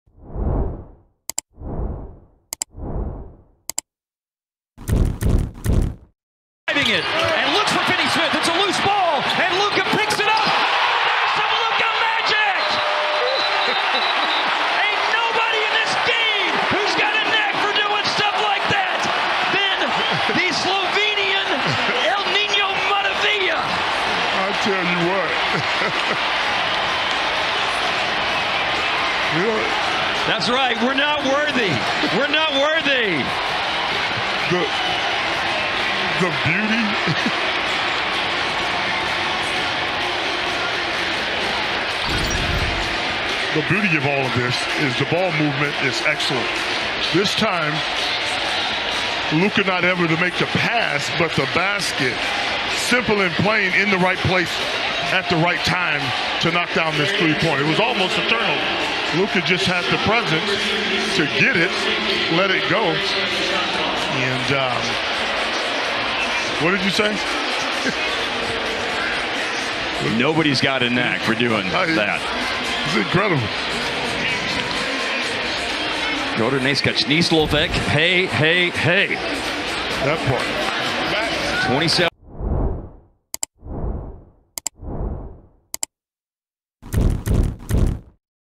LUKA MAGIC-KDOR NE SKACE NI SLOVENC / POTEZA TEKME DALLAS -CLLIPERS
LUKA MAGIC-NI KAJ AMERIKANCI KONCNO VEJO KJE JE SLOVENIJA